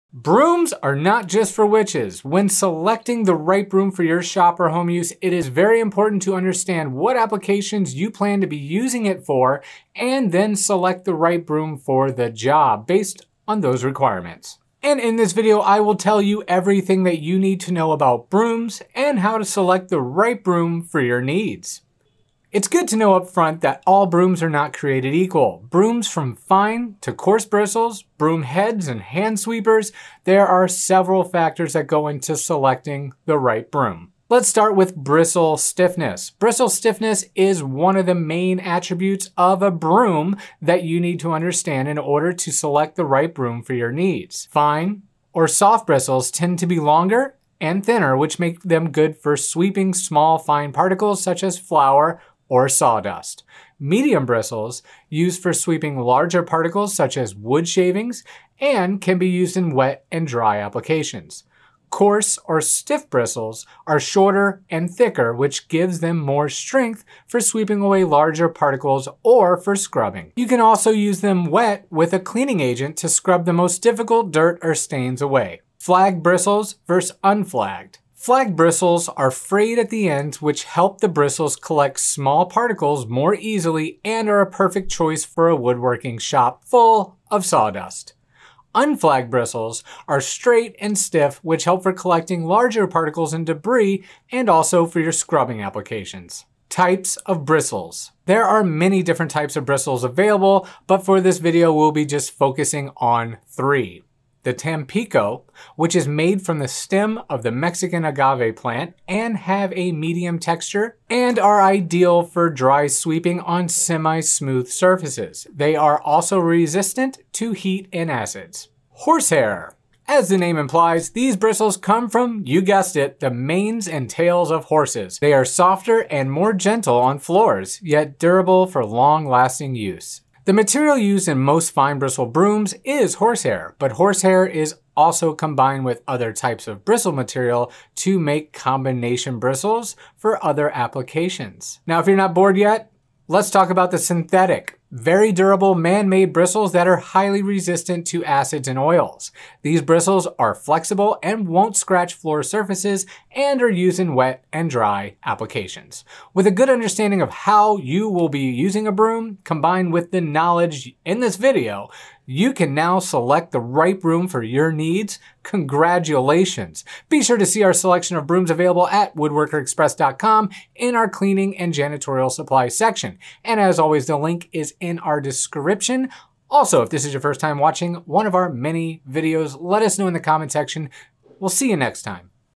Everything you need to know about BROOMS!
Brooms aren’t just for witches, when selecting the right broom for your shop or home use, it’s important to understand what applications you plan to be using it for and then select the right broom for the job based on those requirements. In this video I will tell you everything that you need to know about brooms and how to select the right broom for your needs.
It’s good to know upfront that all brooms are not created equal.
Brooms from Fine to Course bristles….broom heads and hand sweepers
There are several factors that go into selecting the right broom:
 Bristle stiffness
 Flagged vs Unflagged
 Type of bristles

Bristle Stiffness
Bristle stiffness is one of the main attributes of a broom you need to understand to select the right broom for your needs.
 Fine or soft bristles tend to be longer and thinner which make them good for sweeping small, fine particles such as flour or dust.
 Medium bristles – Used for sweeping larger particles such as wood shavings. Used in wet and dry applications.
 Coarse or stiff bristles are short and thicker which gives them more strength for sweeping away large particles or for scrubbing. Use them wet with a cleaning agent to scrub the most difficult dirt away.
Flagged Bristles vs. Unflagged
 Flagged bristles – frayed at the ends which help the bristles collect small particles more easily.
 Unflagged bristles – straight and stiff which help with collecting larger particles and debris and also for your scrubbing applications.
Types of Bristles
There are many different types of bristles available but we’ll be focusing this article on just three:
 Tampico – Made from the stem of the Mexican agave plant and have a medium texture and are idea dry sweeping on semi-smooth surfaces. They are also resistant to heat, alkali, and acids.
 Horsehair – As the name implies, these bristles come from the manes and tails of horses. They are softer and more gentle on floors yet durable for long-lasting use. The material used in most Fine bristle brooms is Horsehair but Horsehair is also combined with other types of bristle material to make combination bristles for other applications.
 Synthetic – Very durable man-made bristles that are highly resistant to acids and oils. These bristles are flexible and won’t scratch floor surfaces and are used in wet or dry applications.
With a good understanding of how you will be using a broom combined with the the knowledge of what are the different types of broom materials, bristle types, and whether you need flagged or unflagged, you can select the right broom for your needs.
See the selection of brooms available at WoodworkerExpress.com in our Cleaning and Janitorial Supplies section